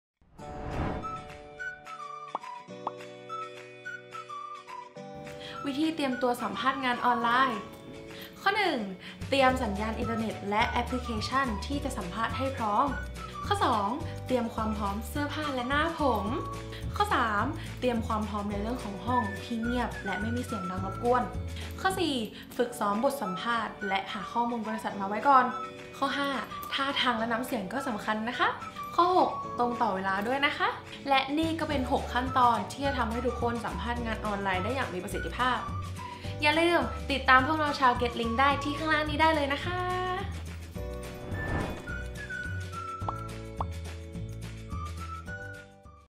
วิธีเตรียมตัวสัมภาษณ์งานออนไลน์ How to prepare yourself when you have an online job interview
✅ใครกำลังสมัครงานอยู่ในช่วง new normal เเบบนี้ Career Advice ก็มาพร้อมกับ 6 ขั้นตอนเเละวิธีเตรียมตัวที่จะทำให้ทุกคนสัมภาษณ์งานออนไลน์ได้อย่างมีประสิทธิภาพ ชมคลิปด้านล่างก่อนไปสัมภาษณ์งานออนไลน์กัน!

📍ลงทะเบียนเเละมาอัพเดทเรซูเม่บน GetLinks ค้นหาโอกาสทำงานที่ดีที่สุดที่นี่!